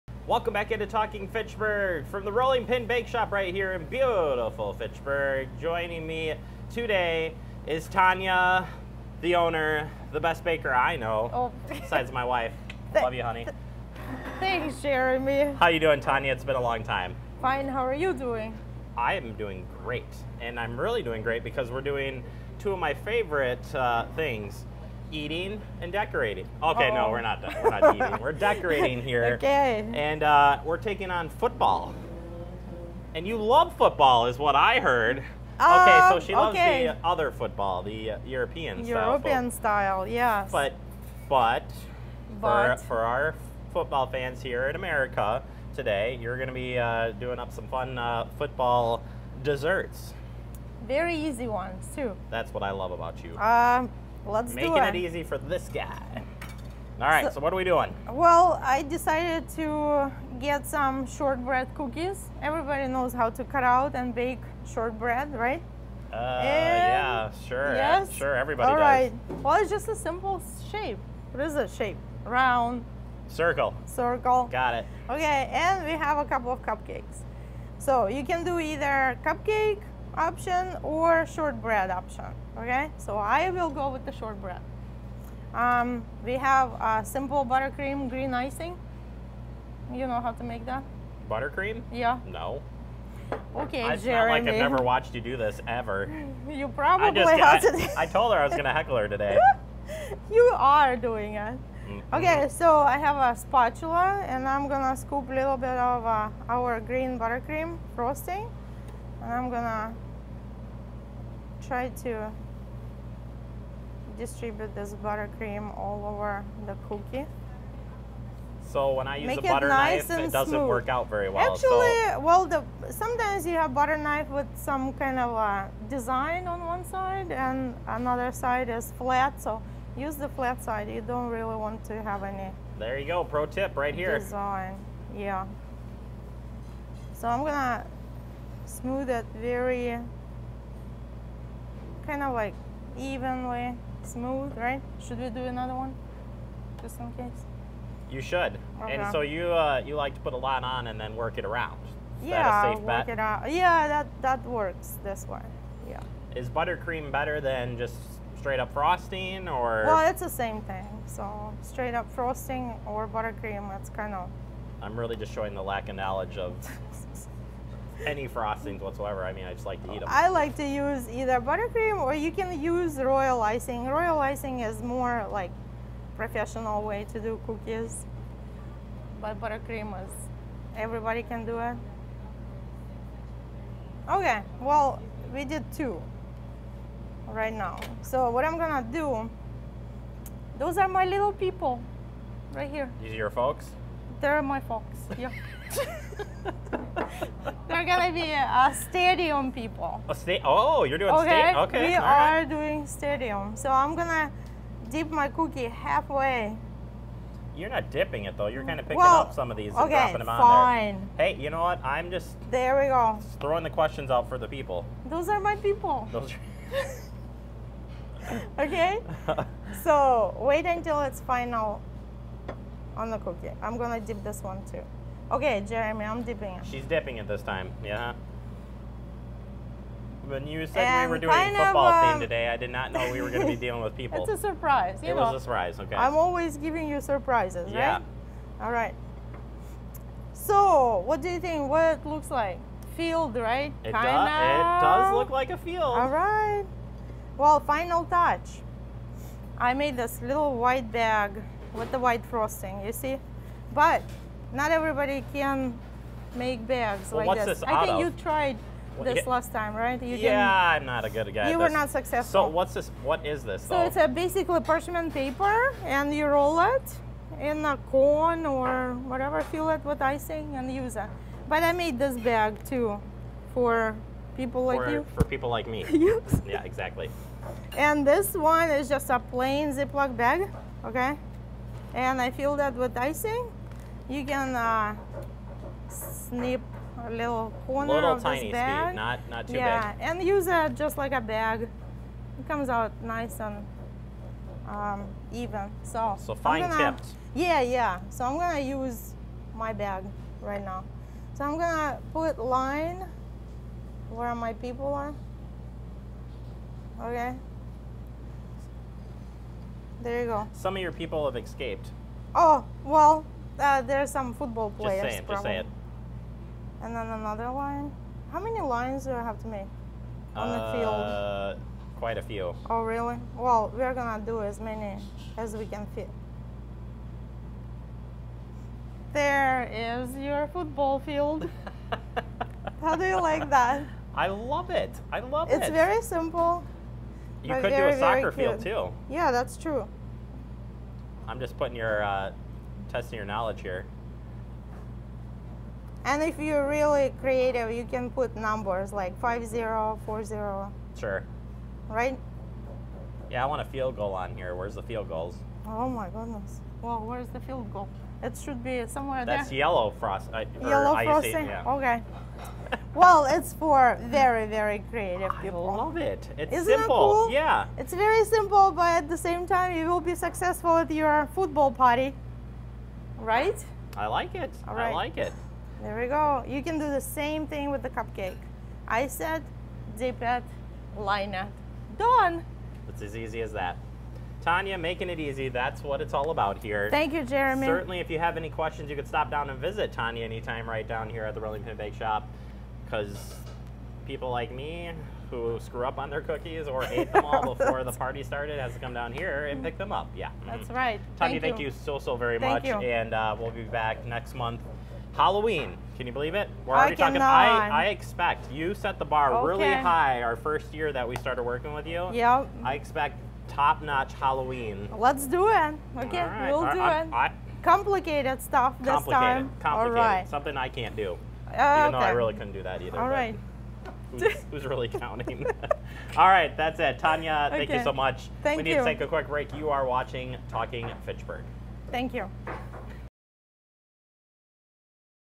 Rolling Pin Bake Shop - Football Cookie 10-4-18
We head over to the Rolling Pin Bake Shop and help Tanya make a football themed sugar cookie.

FACTv